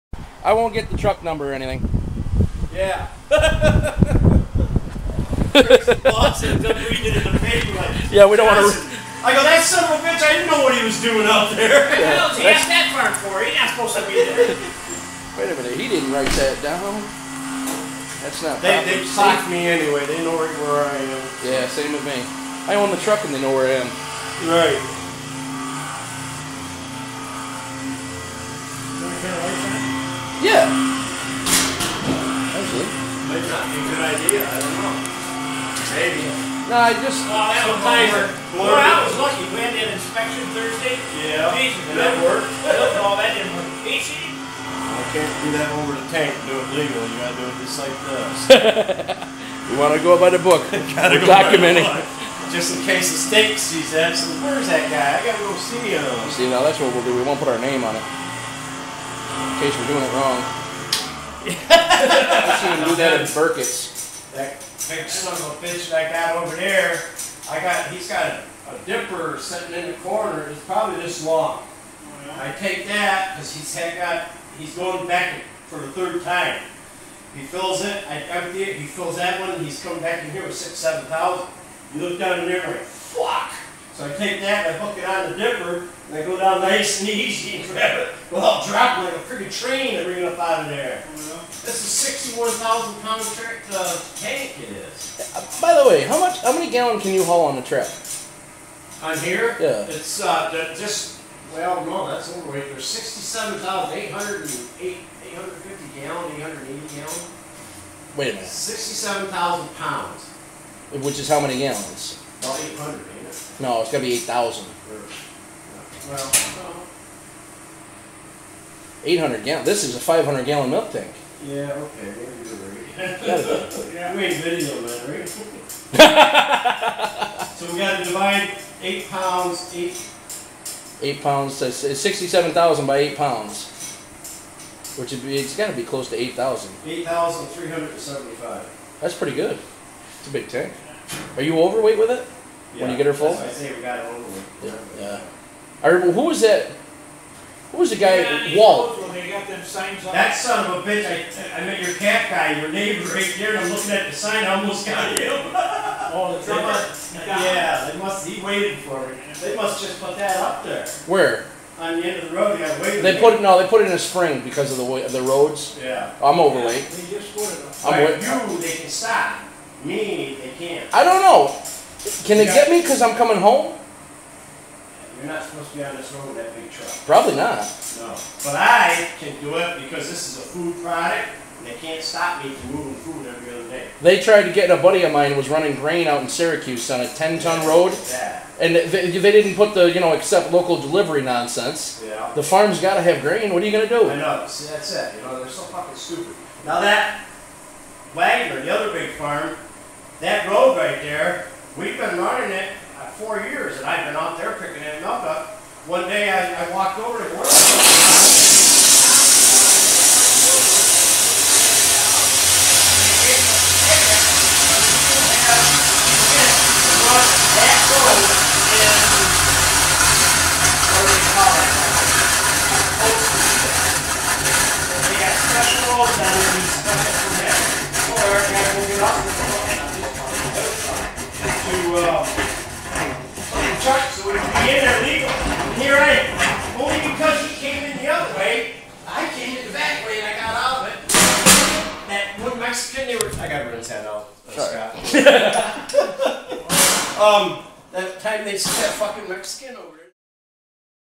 A Visit from the Milkman
A quick look at what happens when the truck driver comes to pick up the milk. After running the vaccum hose and power cord into the barn, he checks the level on the weigh stick and tests the milk. Once the sample is stored he begins sucking the milk out into the truck, which we decided holds about 8000 gallons, here he is getting about 350.